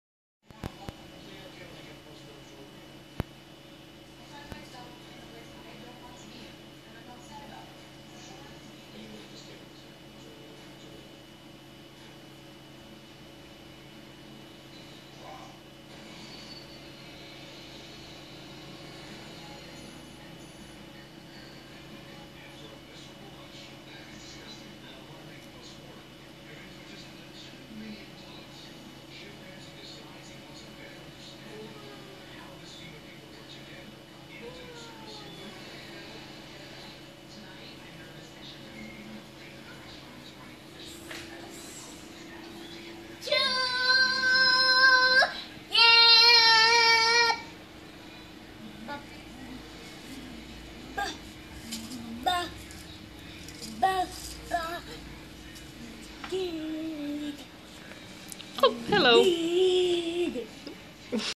Live Action Writing
The Little Duck writes EYE all by himself.